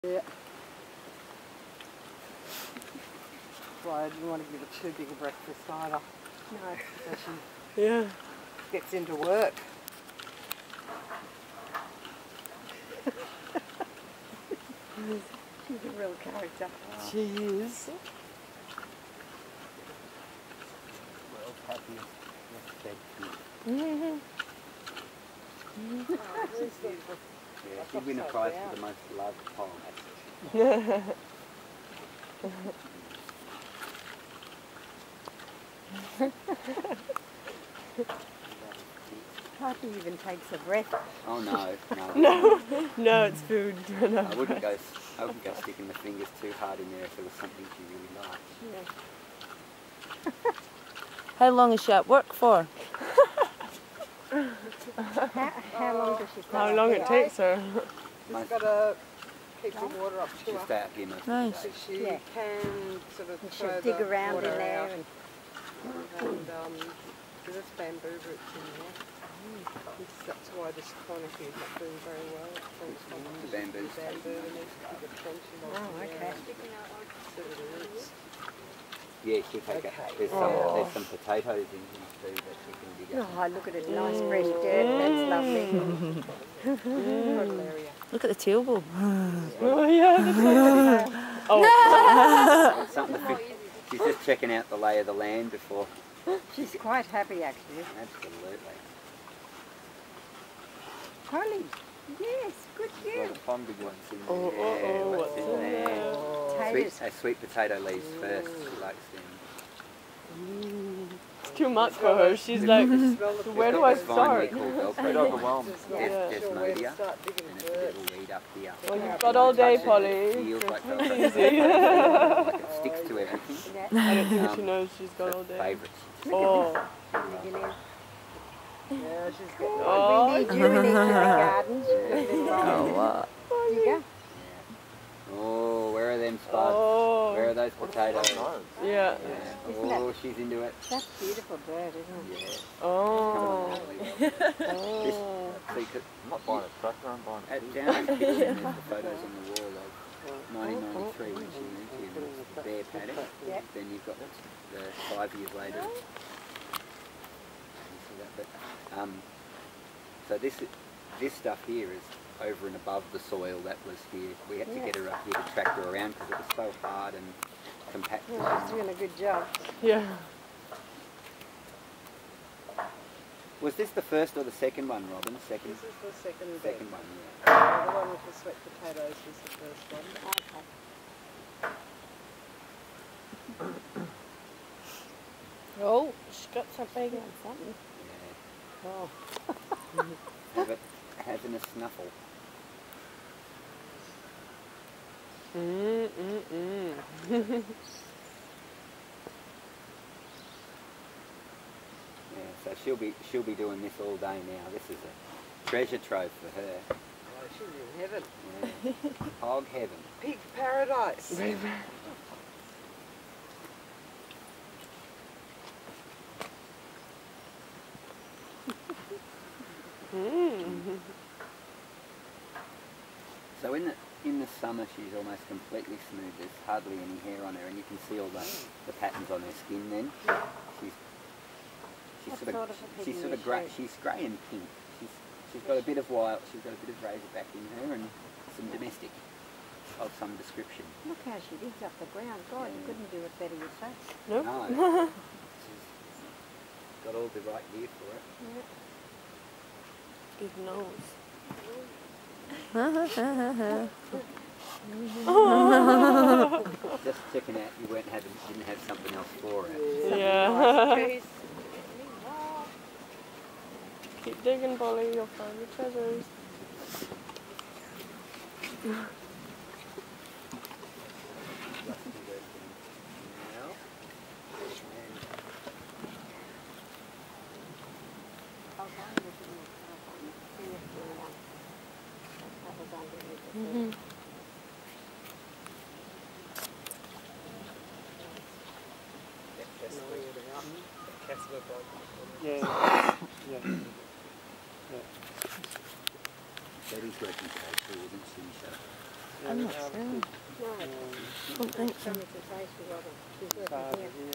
Polly working in her Tractor Pen Djanbung Gardens Nimbin Permaculture College Australia
Permaculture Polly working in her Tractor Pen @ Djanbung Gardens 2014